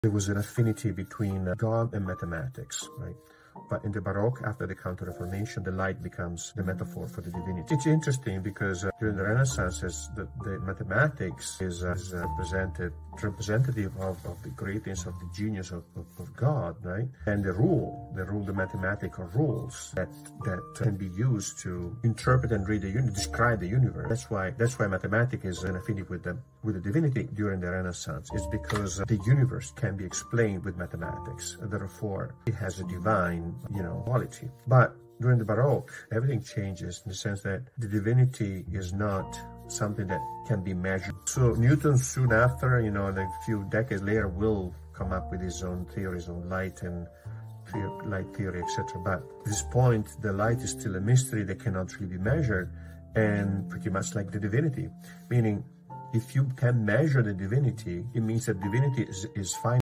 From Divine Math to Mystical Light
From Divine Math to Mystical Light - During the Renaissance, there was a strong affinity between God and mathematics. Mathematics was seen as a representation of God's genius, with mathematical rules being used to interpret and describe the universe. This connection made mathematics seem divine because it could explain the cosmos in an orderly, rational manner.

However, after the Counter-Reformation during the Baroque period, this perspective shifted. Light became the primary metaphor for divinity. Unlike mathematics, light was mysterious and could not be easily measured or quantified. This shift reflected a changing view of divinity as something beyond human comprehension. Though Newton would later develop his theories on light, during the Baroque era, light remained a symbol of the unmeasurable and transcendent nature of the divine.